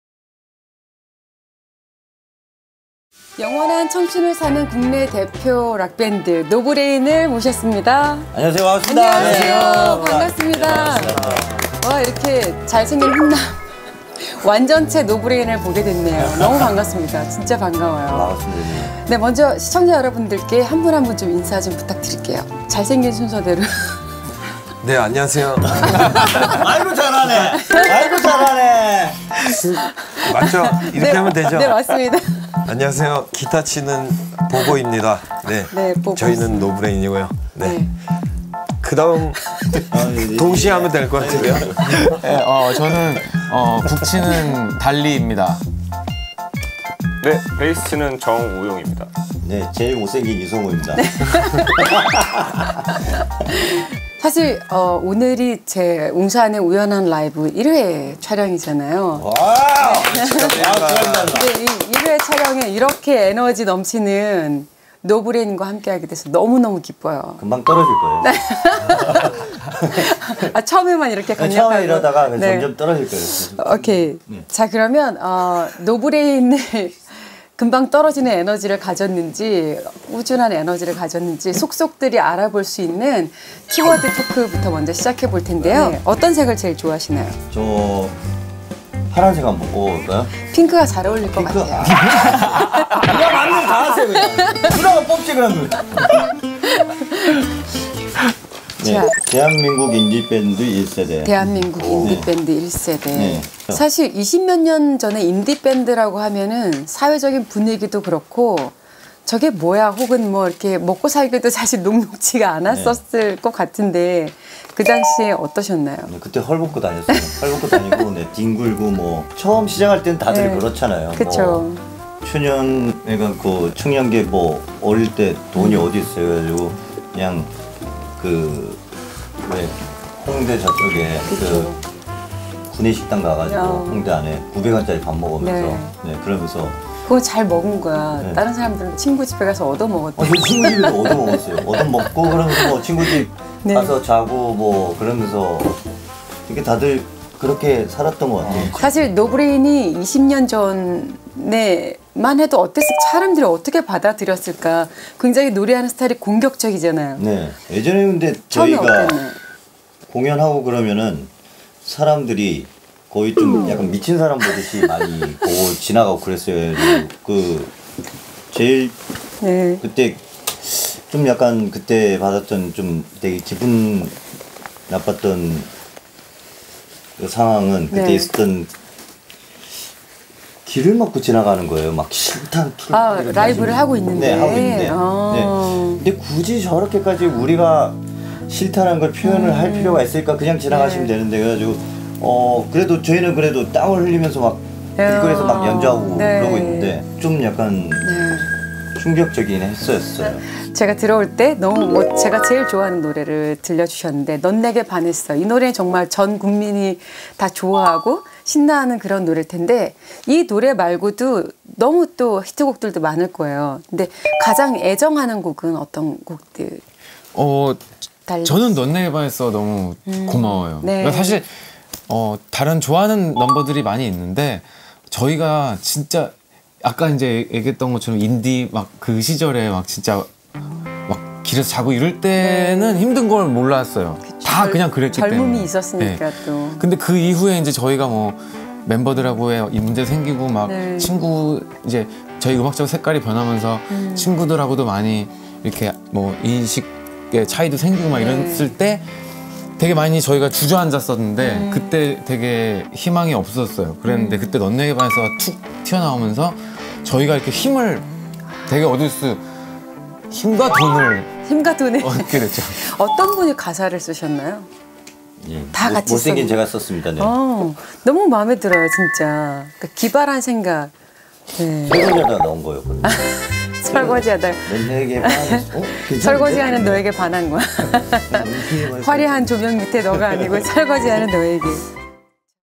[웅산의 우연한 라이브] 노브레인 - 키워드 토크1: 인디밴드 1세대
[웅산의 우연한 라이브]
인디밴드 1세대를 대표하는 노브레인, 그들의 이야기를 키워드를 통해 알아보는 시간.